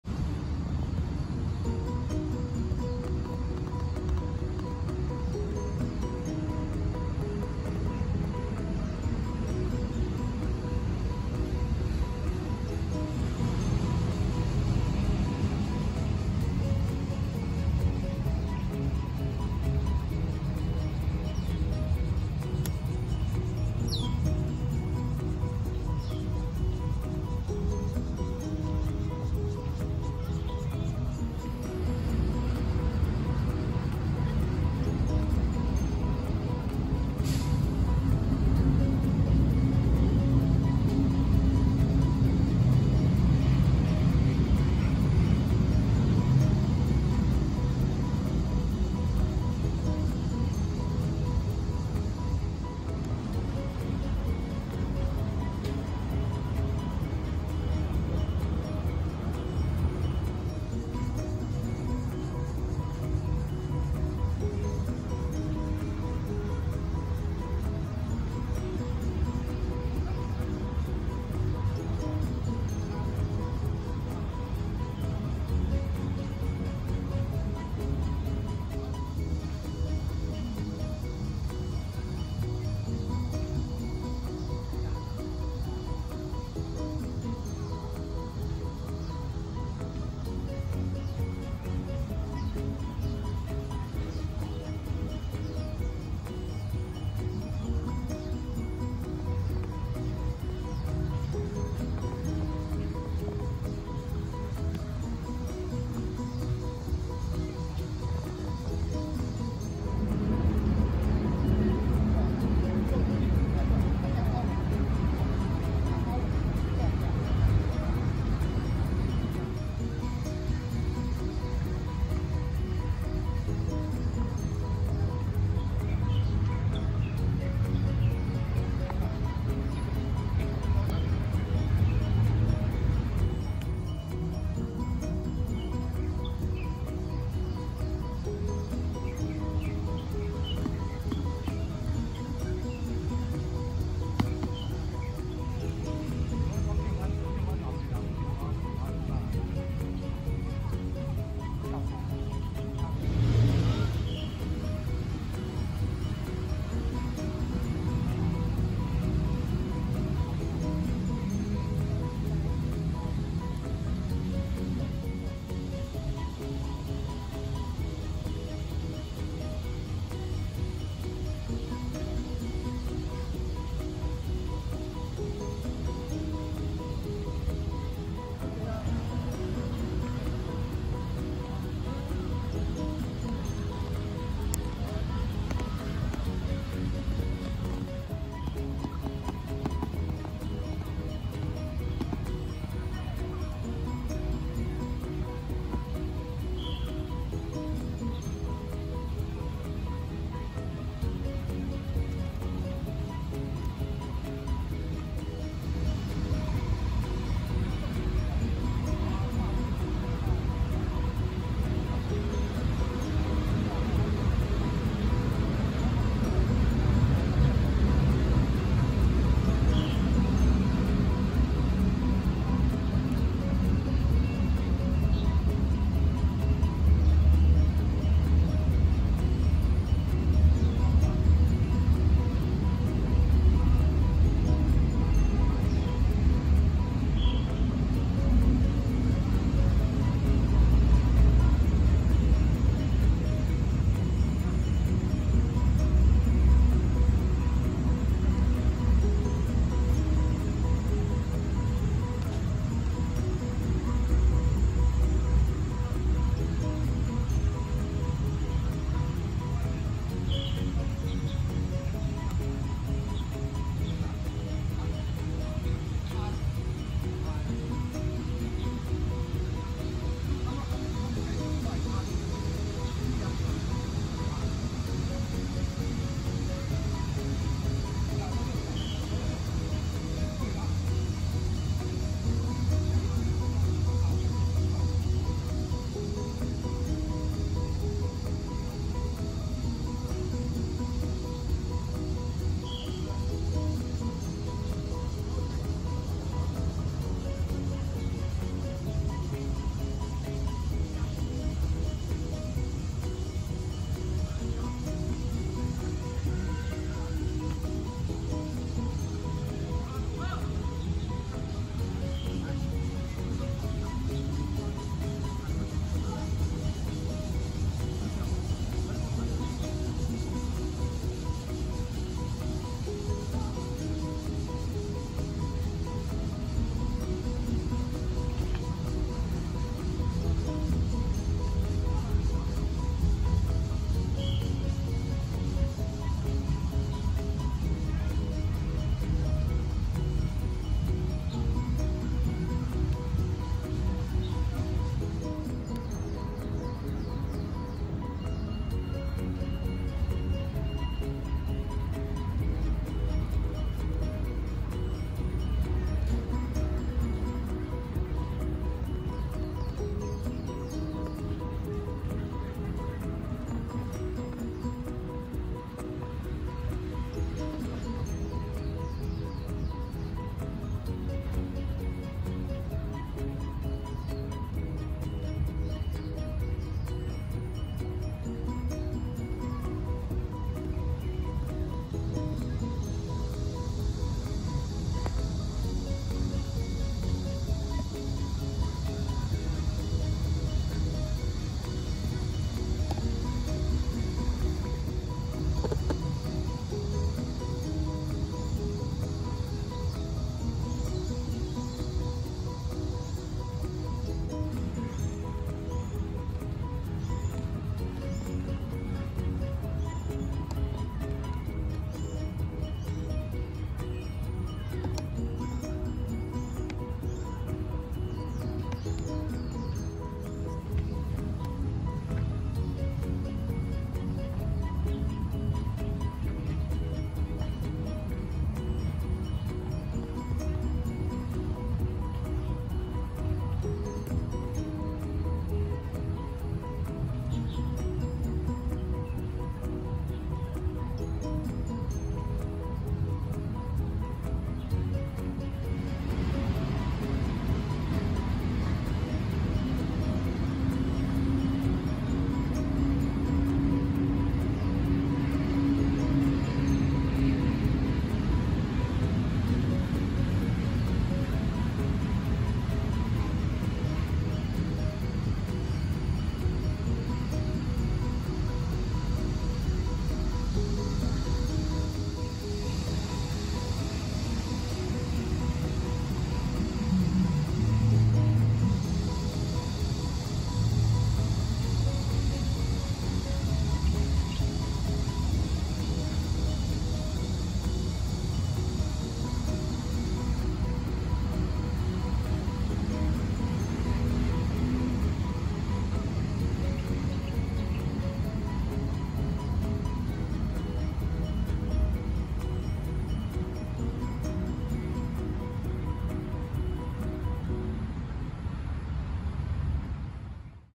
20230702「榮星花園的百年老黃槿樹」分享許雅智的手機視頻
片長：10分鐘21秒
拍攝現場：台北市中山區榮星花園公園4號出入口附近